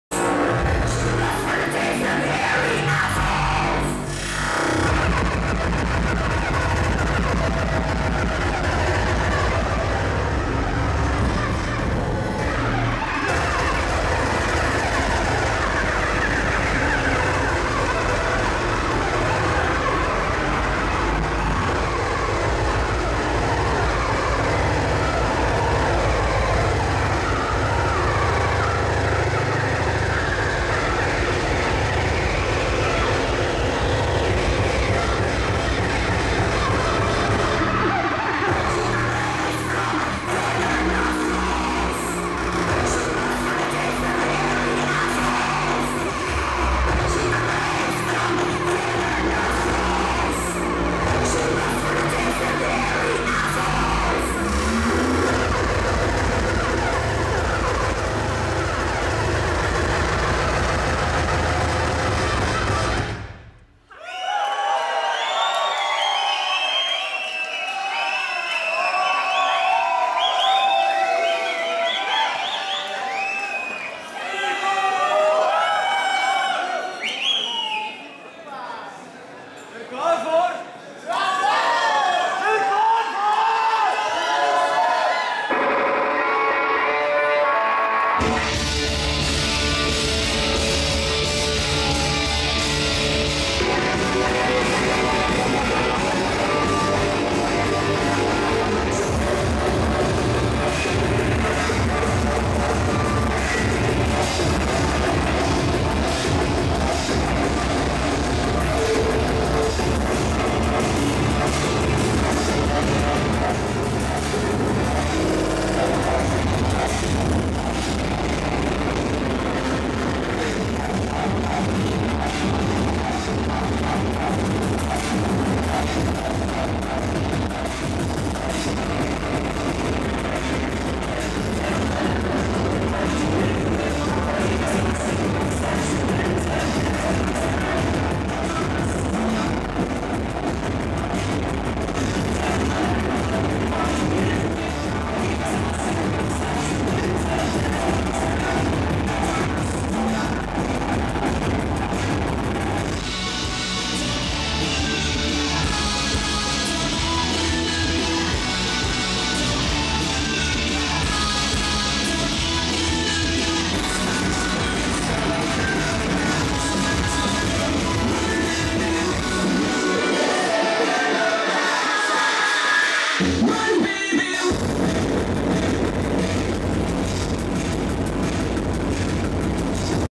BGMW 7th an Valav and Sickboy
Breakcore Gives Me Wood 7th Anniversary (23/10/2009)
Valav ending her set, Sickboy beginning his